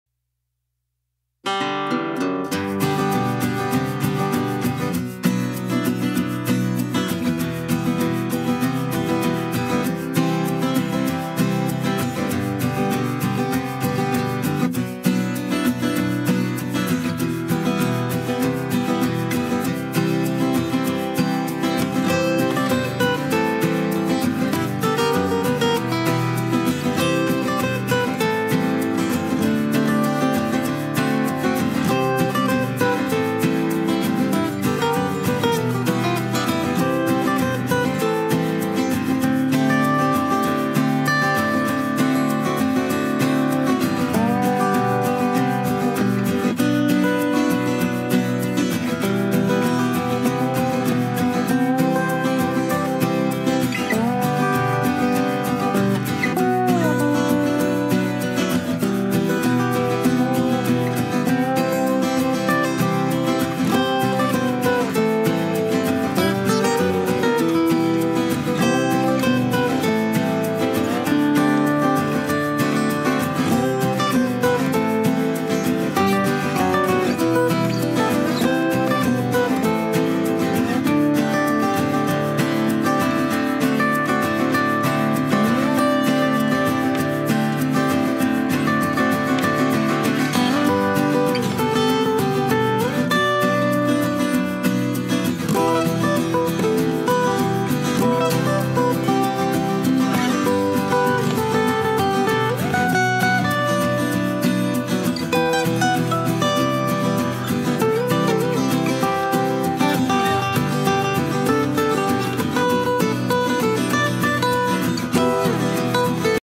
Write notice for all members of student Council to attend meeting to prepare plan forannual function
Q .You are the school captain of the Government Boys Higher Secondary School Jabalpur write a notice for all the other members of the student Council to attend the meeting  to prepare a plan for annual function.